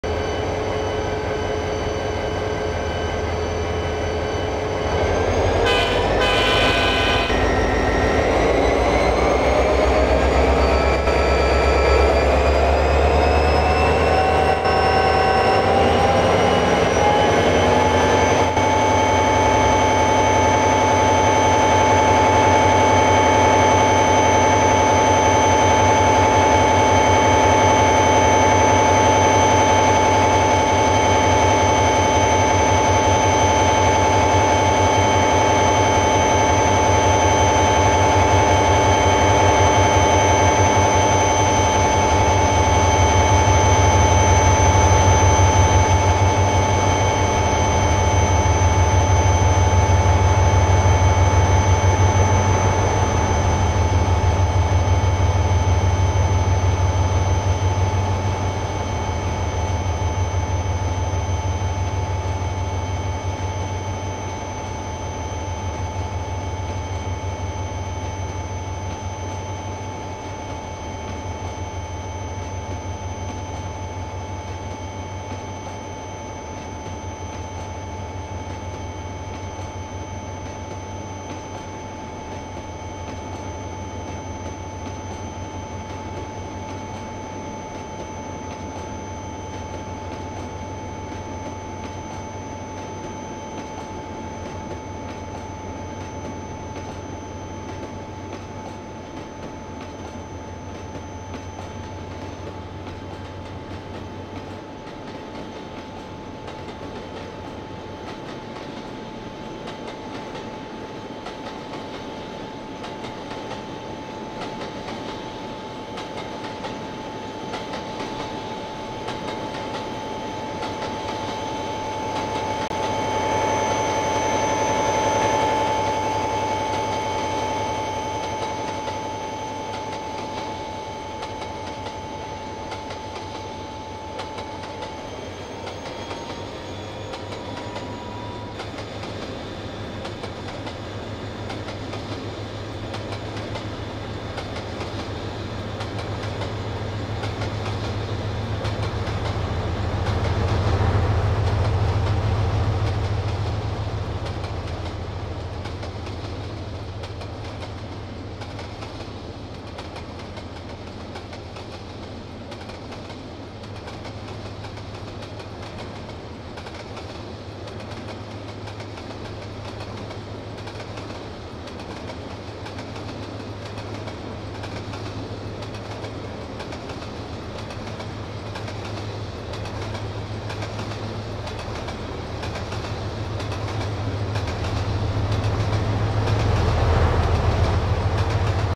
Union Pacific Mixed freight train.MONSTER!!!!! 12 Locs. HD!!!
This is my Union Pacific freight train. For this monster train almost all cars are downloaded, 12 locs!
What do you guys think off this freight train???